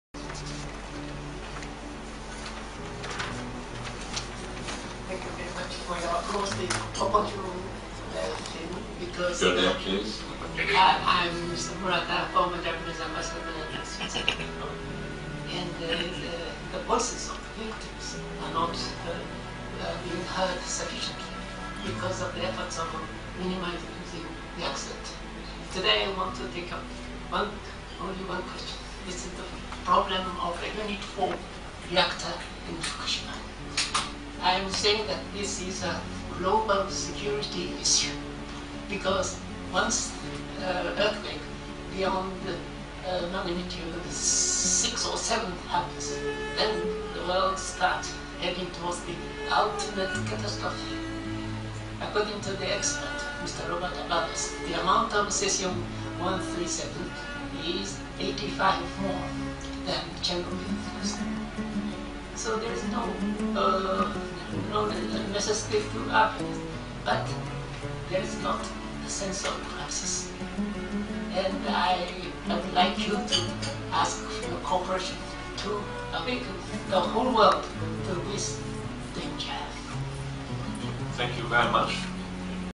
Mitsuhei Murata, Former Japanese Ambassador to Switzerland and Senegal: Statement on Fukushima
Ambassador Murata makes public statement in English: Once an earthquake beyond magnitude 6 or 7 happens, then the world starts heading towards the ultimate catastrophe — Fukushima Unit 4 a global security issue

"I'm Mr. Murata former Japanese ambassador to Switzerland.

The voices of the victims are not being heard sufficiently because of the efforts of the minimizers of the accident.

Today I want to only pick up one question, the problem of the Unit 4 reactor at Fukushima.

I'm saying this is a global security issue.

Because once an earthquake beyond a magnitude 6 or 7 happens, then the world starts heading towards the ultimate catastrophe.

According to the expert Robert Alvarez, the amount of cesium-137 is 85 times more than Chernobyl.

[Garbled Speech], but there's not the sense of crisis.

I would like to ask your cooperation to alert the whole world to this danger."

Hibakusha Press Conference
Foreign Correspondent's Club Japan (FCCJ) in Tokyo on
22 May 2012